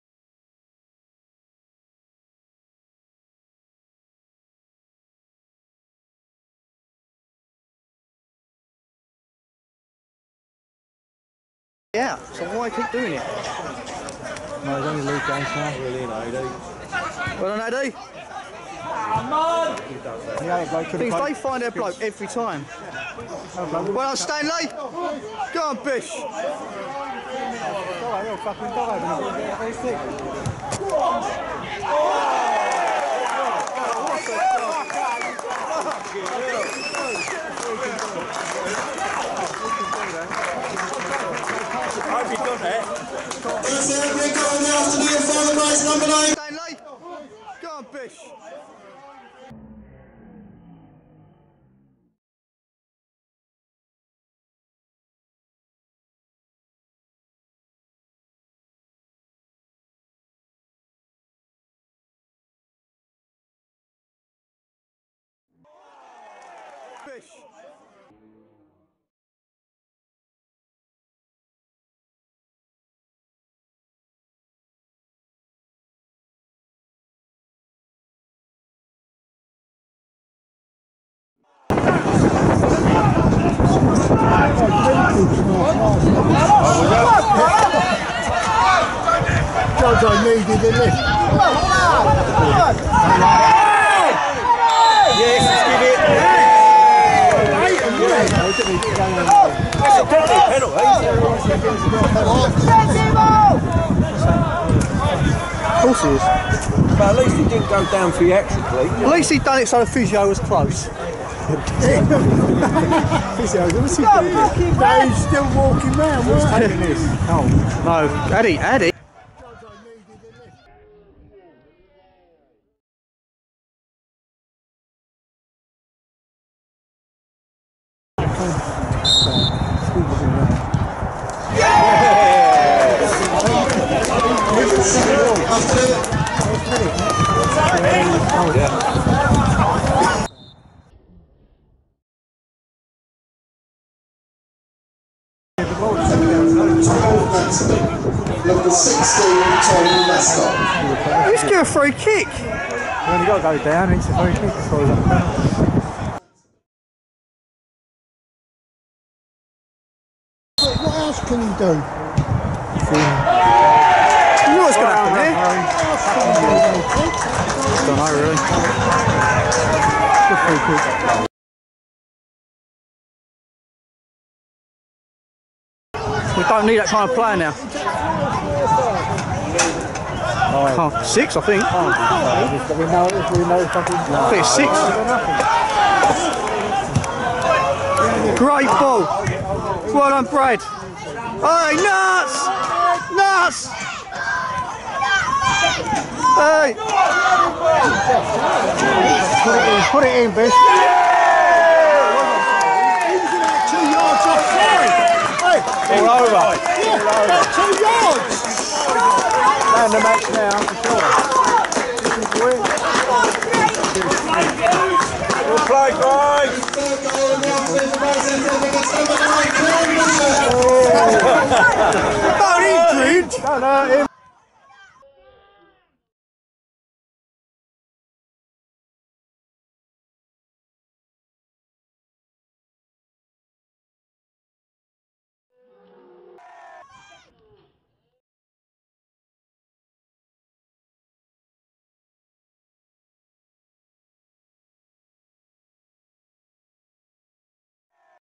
Goals From Grays Athletic v AFC Sudbury 13/1/18
Grays win 3-1 with a worldie From Kieran Bishop and pen from Ade cole.
a good free kick for Sudbury by Billy Holland and finished off on the 90 min by Kieran second off the game